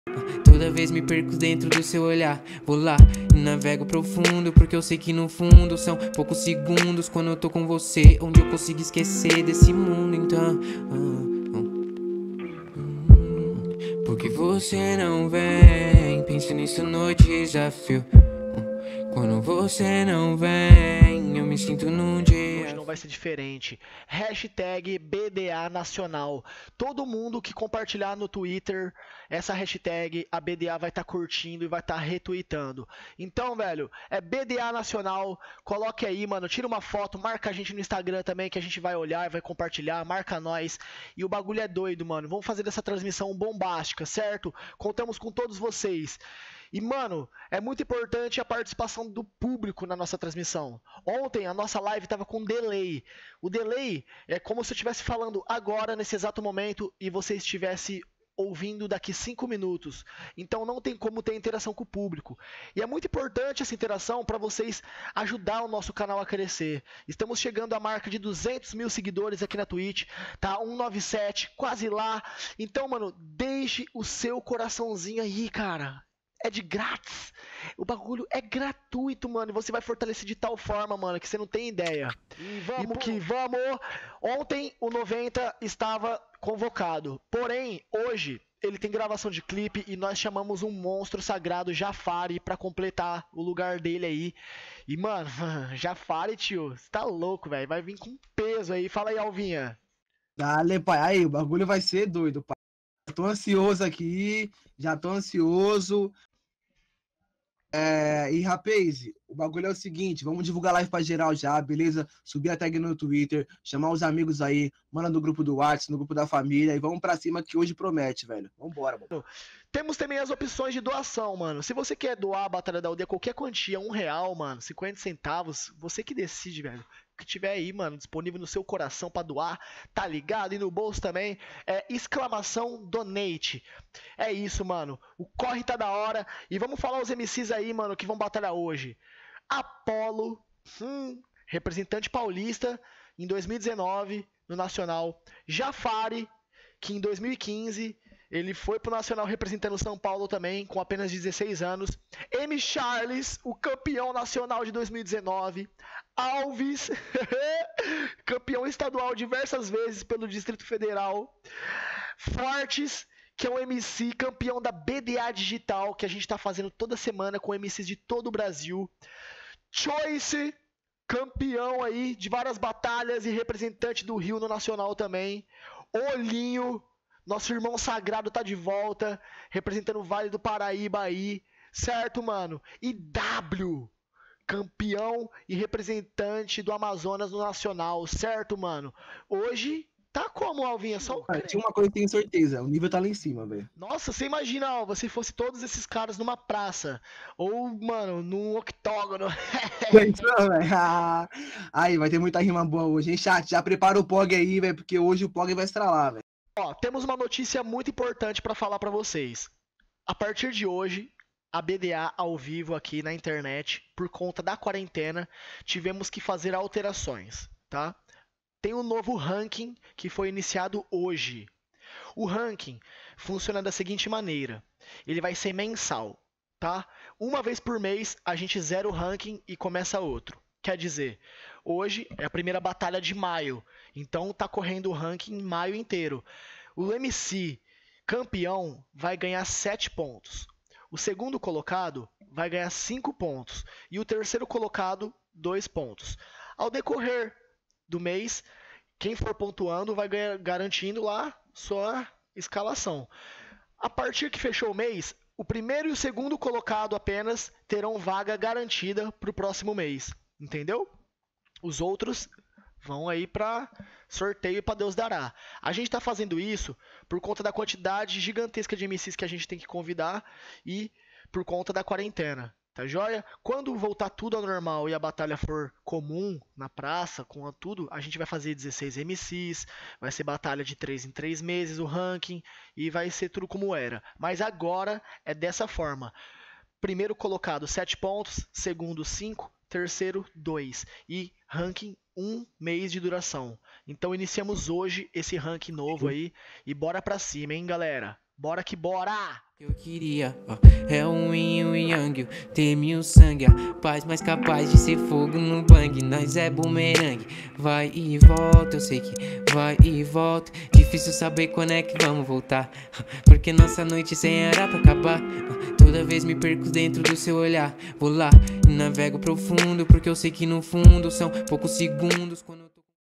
ABERTURA | 191ª Batalha da Aldeia
O novo som do Andrade sai na Sexta feira pelo @AldeiaRecords  fiquem ligados!

Salve galera, devido a limitação de grandes públicos e aglomerações realizamos a edição 191 pelo nosso canal da Twitch; Alcançamos a meta de 5 mil pessoas online assistindo a batalha ao vivo, não perca a próxima transmissão e participe das votações.